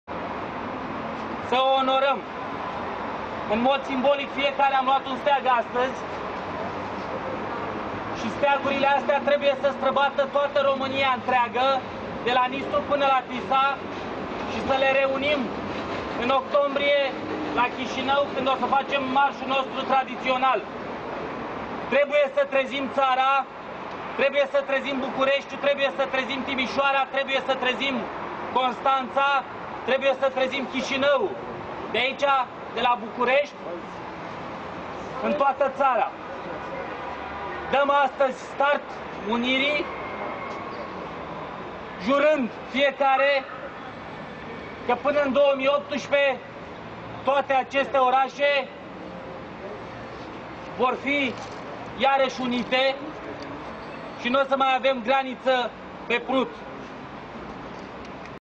Flash-mob organizat de Platforma Unionistă Acțiunea 2012
George Simion: Dăm start Unirii, jur&#226;nd fiecare că p&#226;nă-n 2018 nu vom mai avea graniță de Prut!
Titlul evenimentului: Flash-mob organizat de Platforma Unionistă Acțiunea 2012